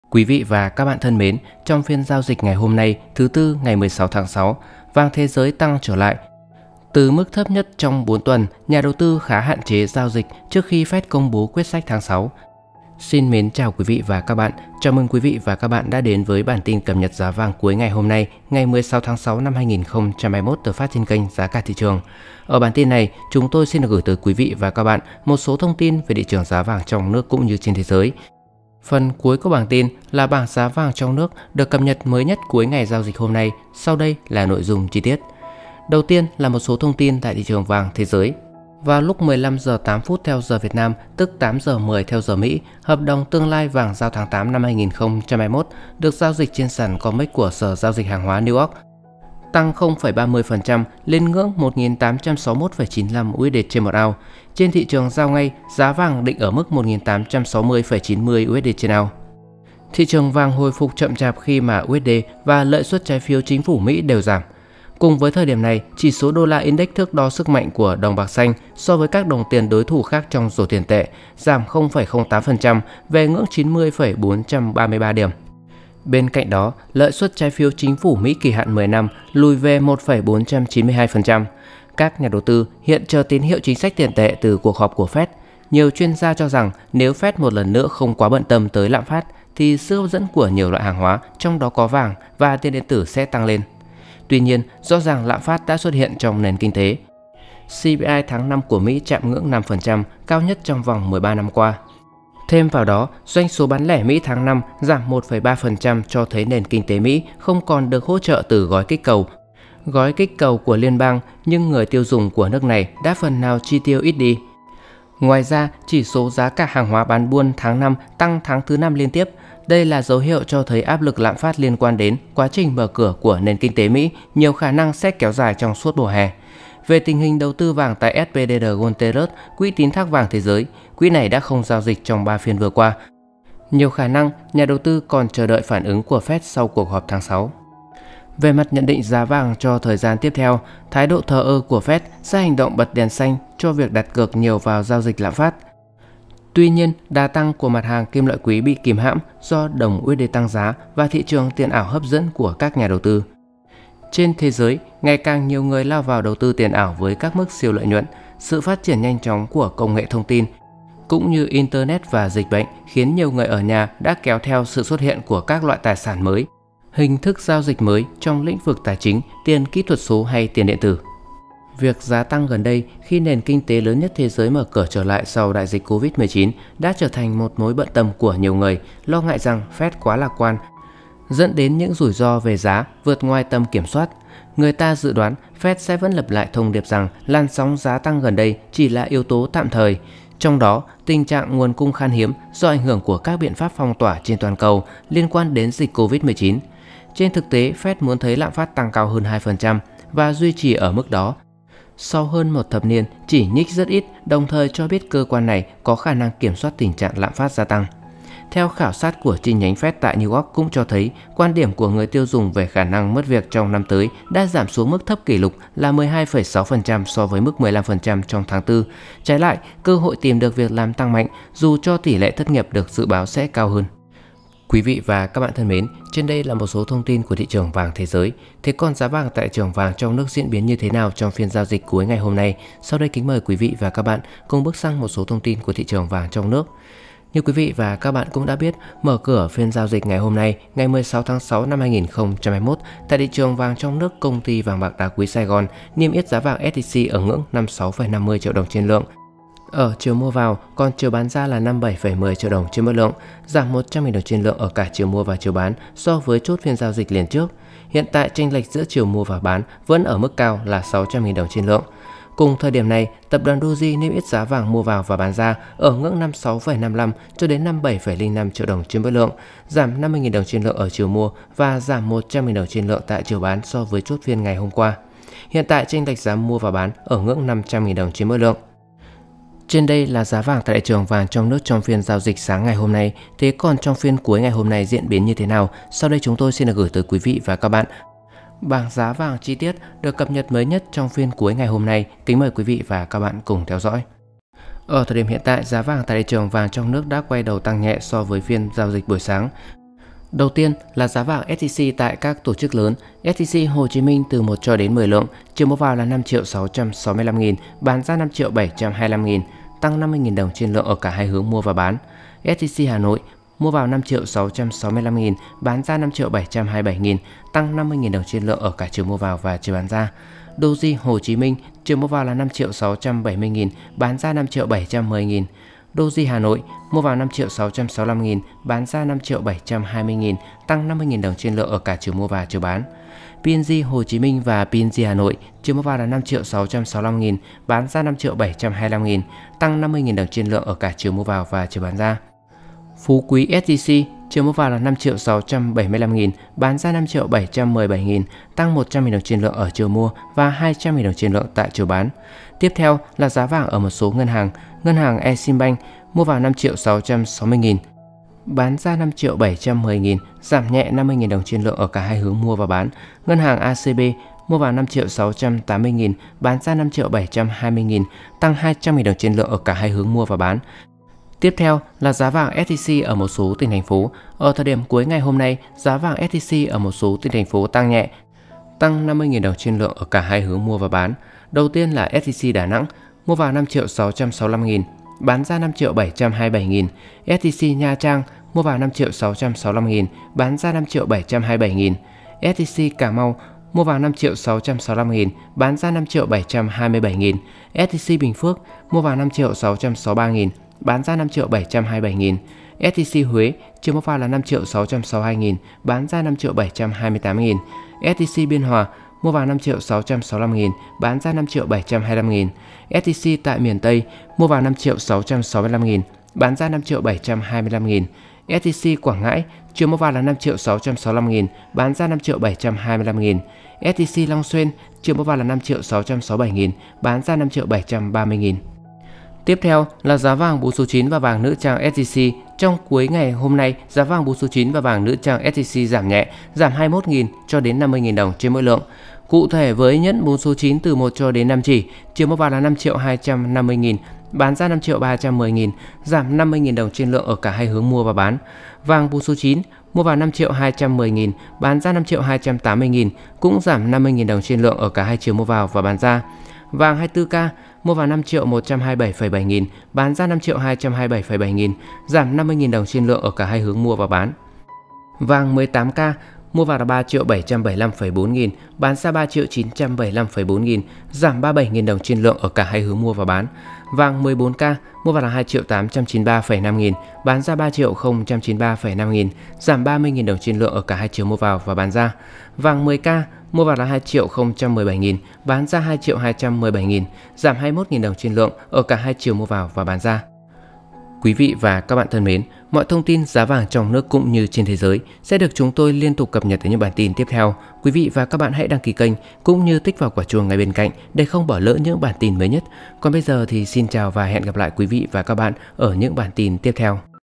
Giá vàng tối nay 9999 ngày 16/6/2021 | TIẾP TỤC CHỊU LỖ || Bảng Giá Vàng SJC 9999 24K 18K 14K 10K
bảng giá vàng hôm nay, giá vàng hôm nay ngày 17 tháng 6, giá vang 9999 hom nay, giá vàng hom nay